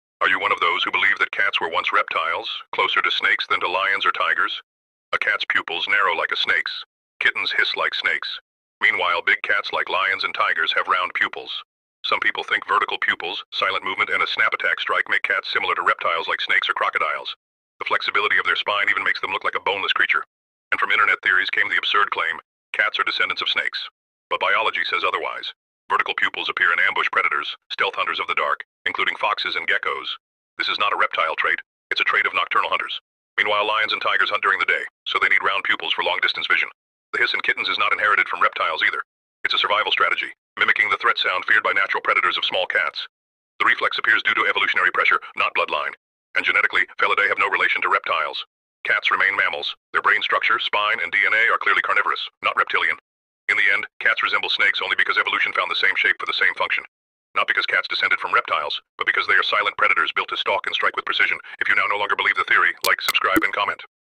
The Truth Behind Cat Eyes
Many people believe vertical cat pupils mean they evolved from snakes, but biology says otherwise.
Vertical pupils are a trait of ambush predators, not reptiles — shared by foxes and geckos hunting in low light.
Big cats like lions and tigers have round pupils because they hunt under the sun, needing long-distance clarity.
Hissing in kittens is a survival mimicry, not a reptilian inheritance.
Genetically, cats are pure mammals — their bones, brains, and DNA have no reptile connection at all.